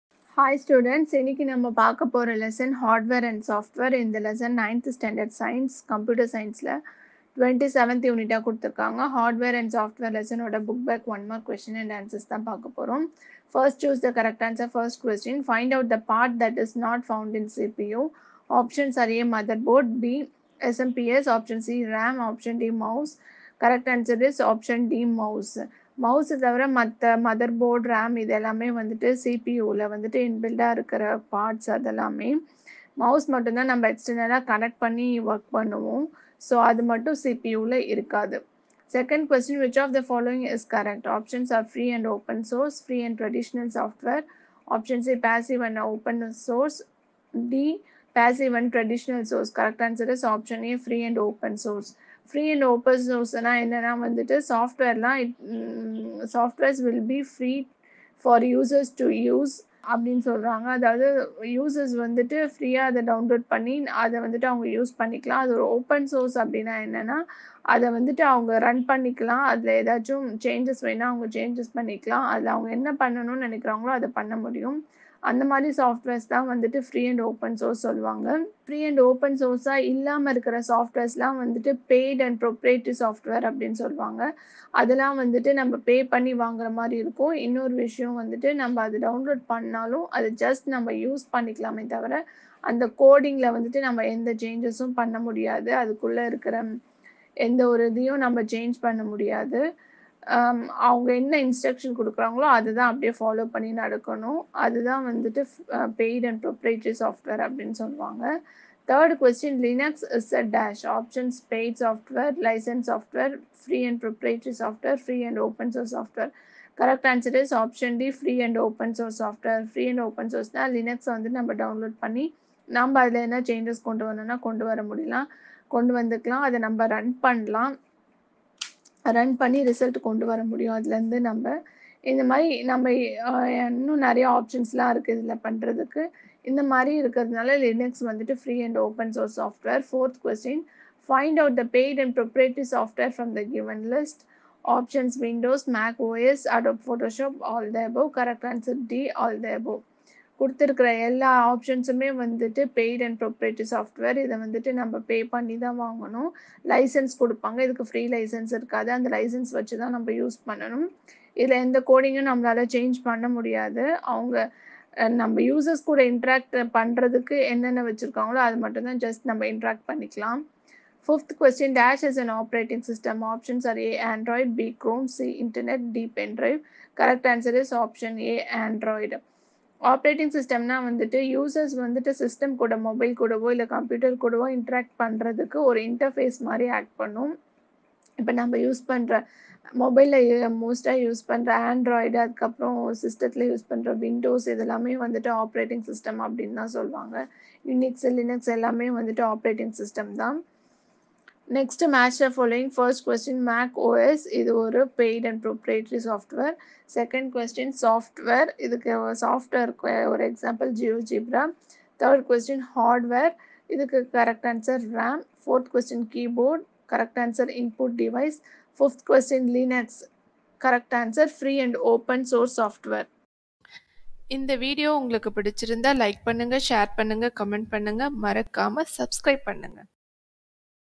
Visual Communication Book Back Questions, Answers | Unit 23 | Class 10 | Science | Samacheer Kalvi
Visual Communication Book Back Questions & Answers, Exercises, Solutions, Tests, Quizzes, Videos and Podcasts for Class 10 | Unit 23 |  Science | Samacheer Kalvi - English Medium

by EduDeck. Learn, Practice & Evaluate From Anywhere.

00:00 Introduction
00:14 Choose the correct answer
03:22 Match the following